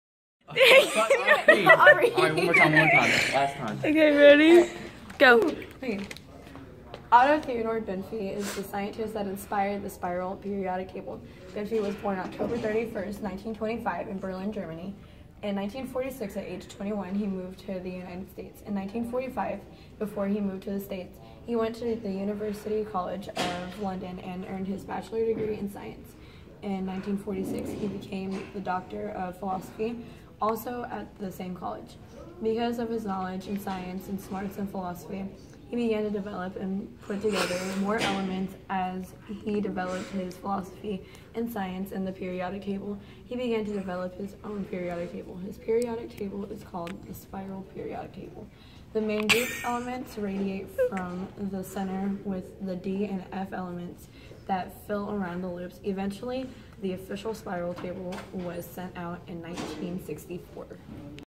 Otto Theodor Benfey
Facts about Benfey and his spiral periodic table